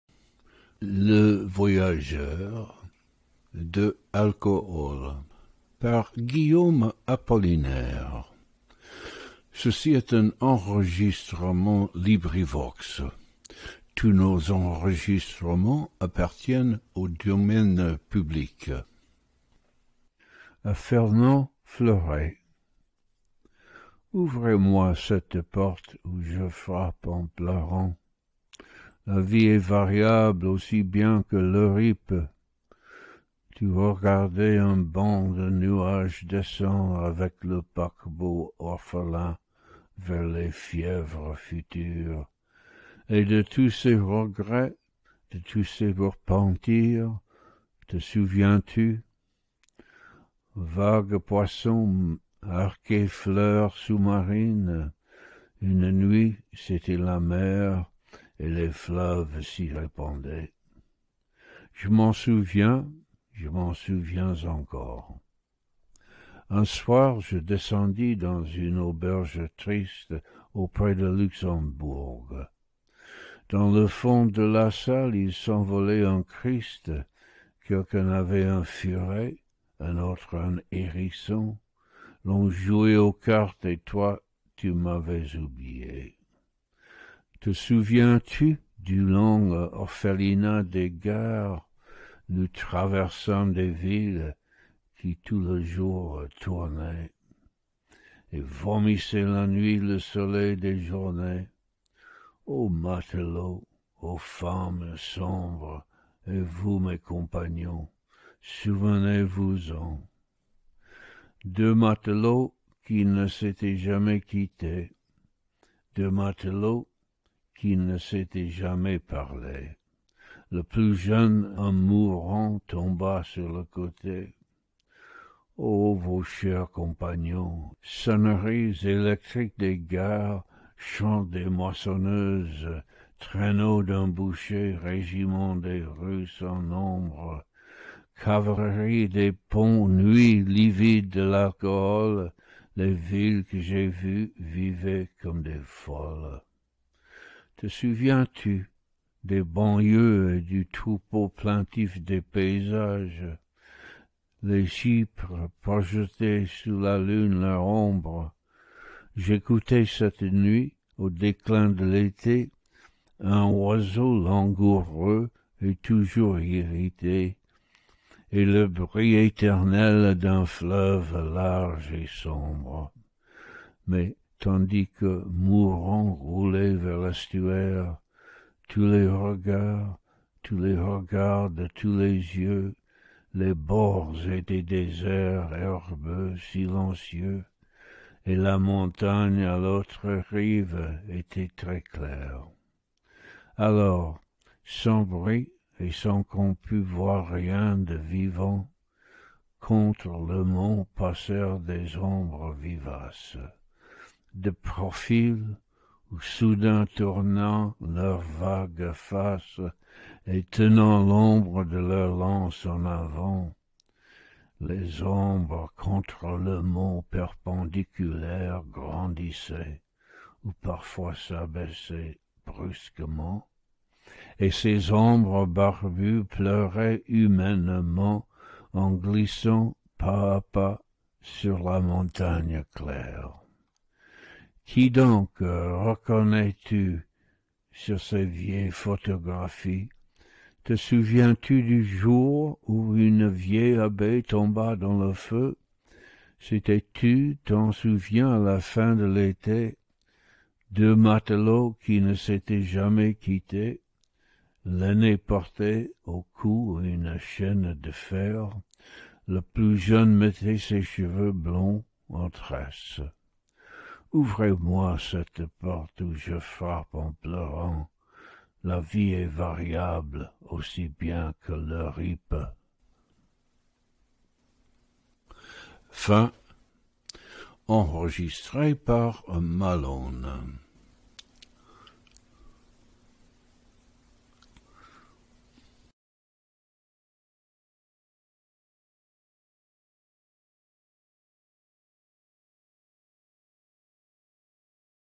Alcools by Guillaume Apollinaire 13 - Le voyageur Full Audiobook
Paperback (paid link)

Alcools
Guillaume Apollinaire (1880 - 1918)

Apollinaire is a pivotal figure in the history of French poetry. Friend of Picasso, albeit a sometimes volatile one, inventor of the term 'surrealism' and the poem without punctuation, he advocated a poetry that was direct and intuitive, free of any refined intellectualism. Alcools, published in 1913, represents his most significant collection of poems. (Résumé par Malone)

Genre(s): Poetry

Language: French